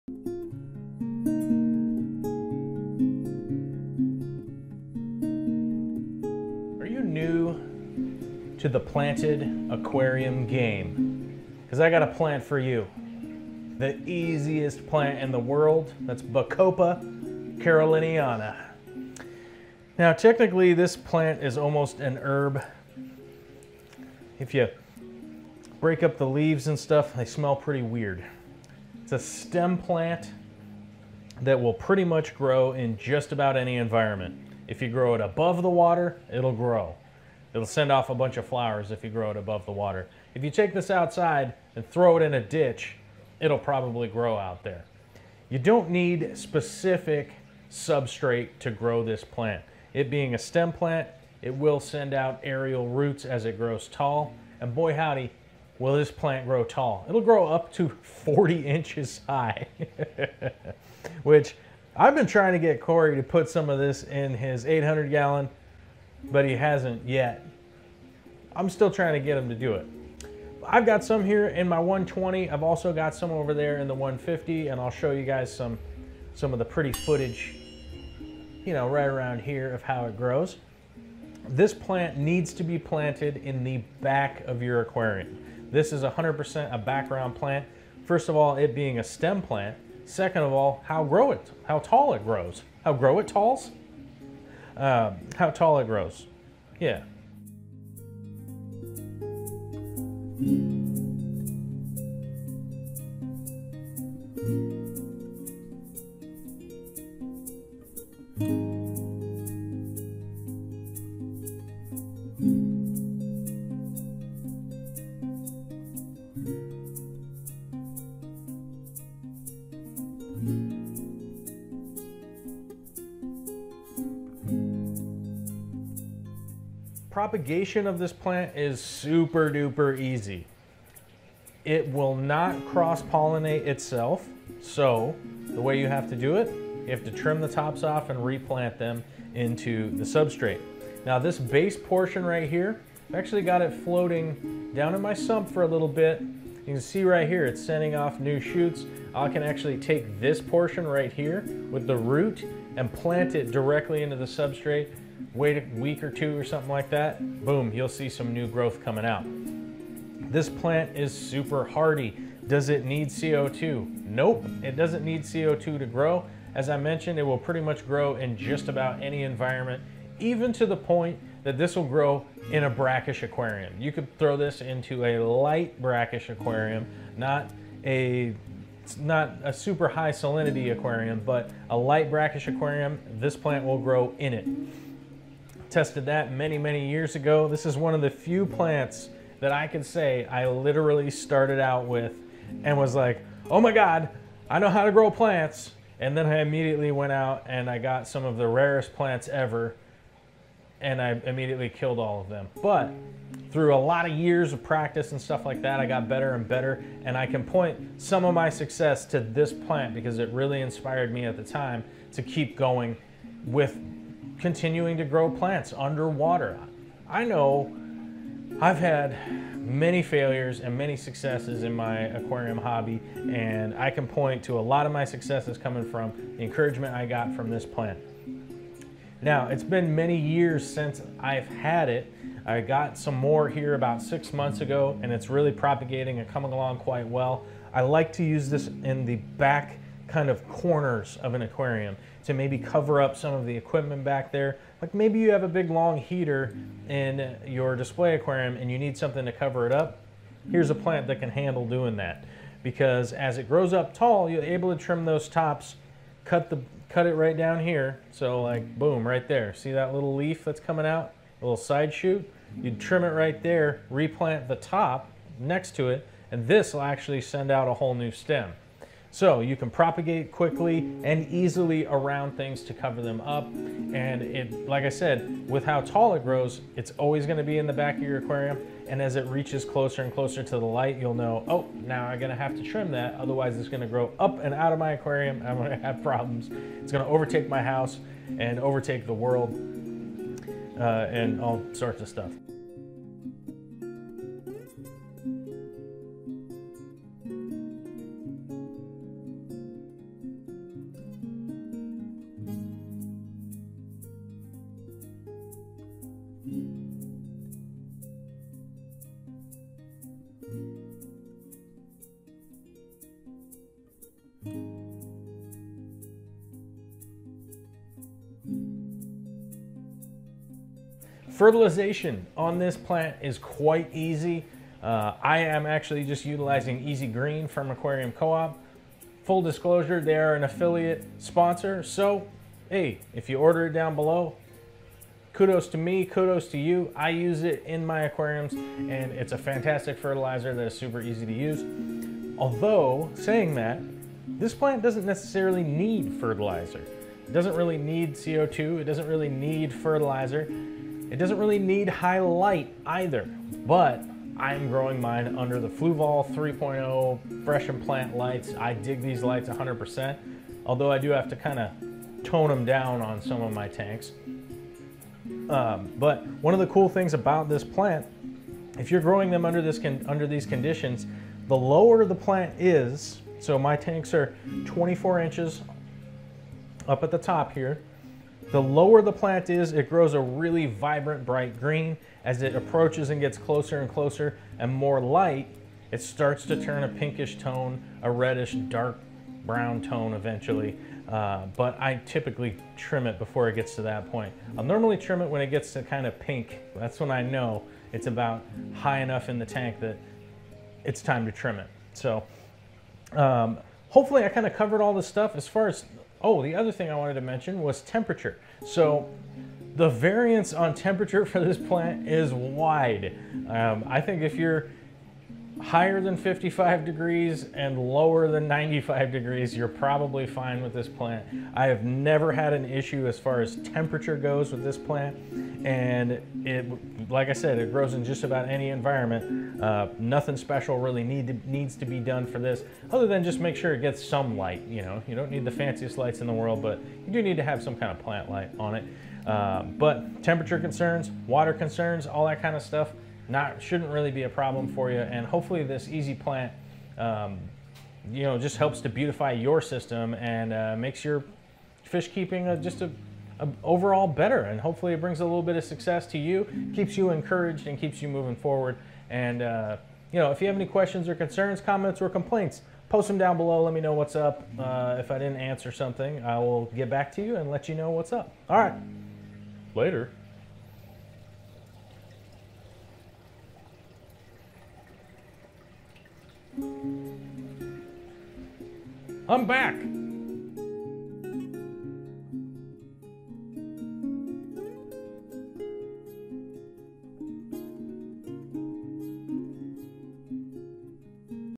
Bacopa Caroliniana Aquarium Plant
Let's take a look at one of the easiest aquarium plants in the hobby, Bacopa Caroliniana!

Amazon Store